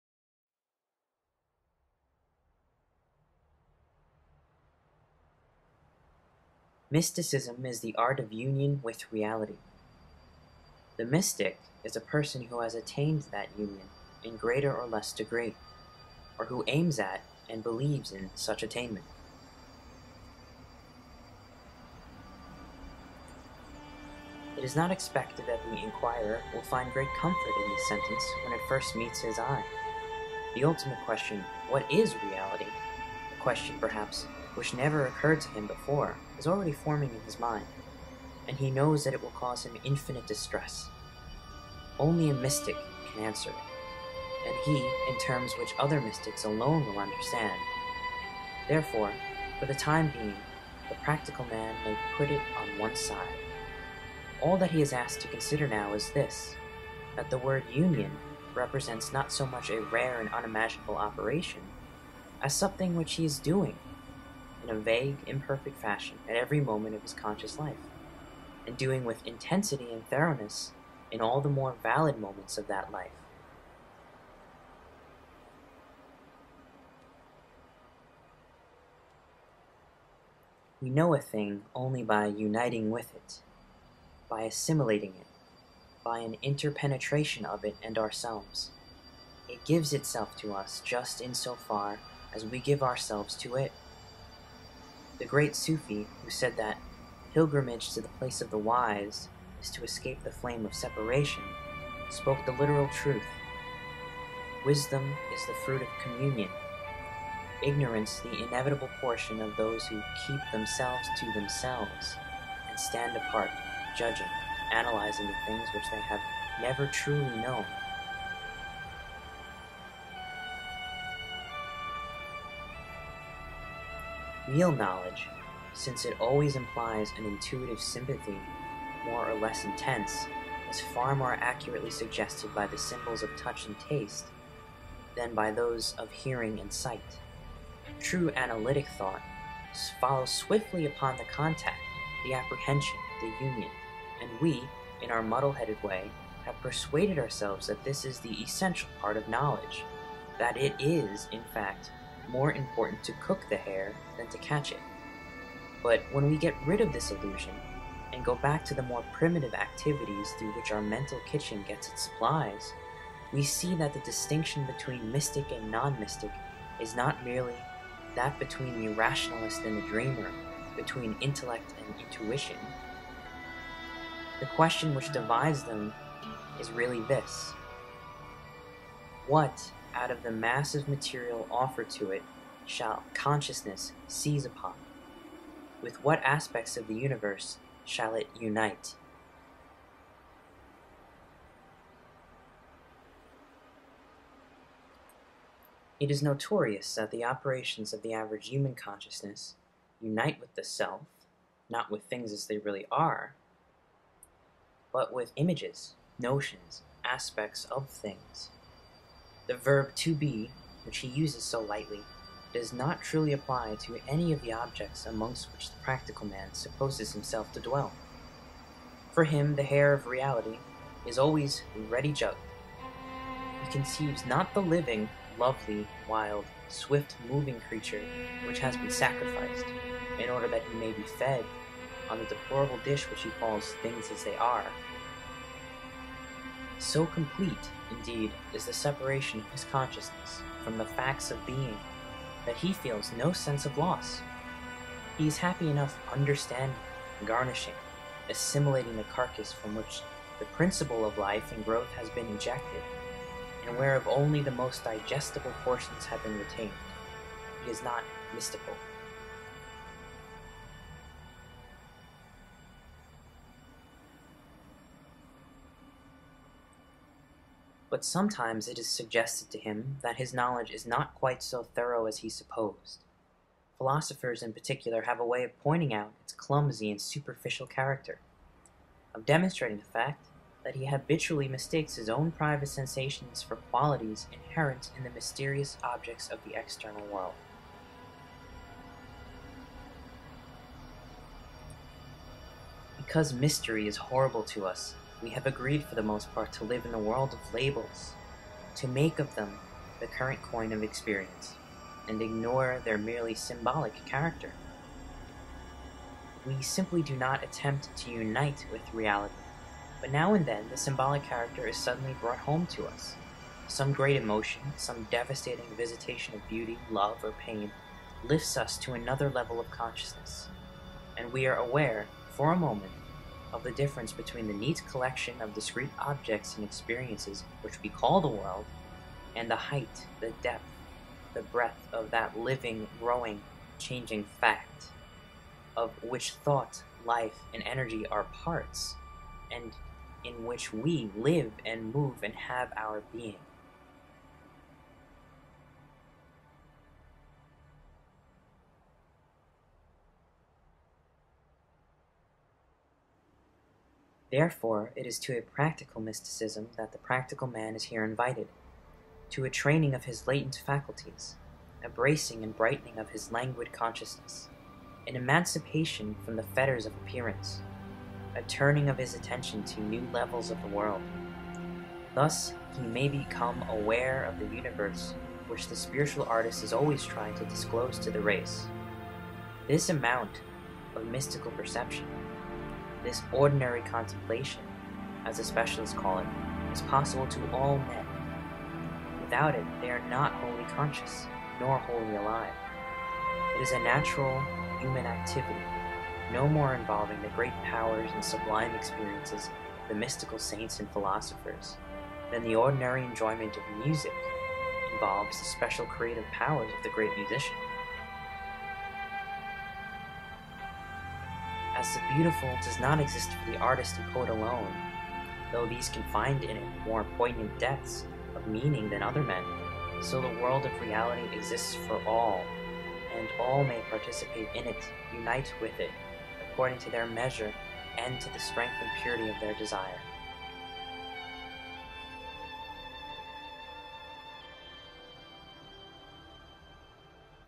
Practical Mysticism
Reading from Practical Mysticism, by Evelyn Underhill.

For further descriptions from the author on contemplation!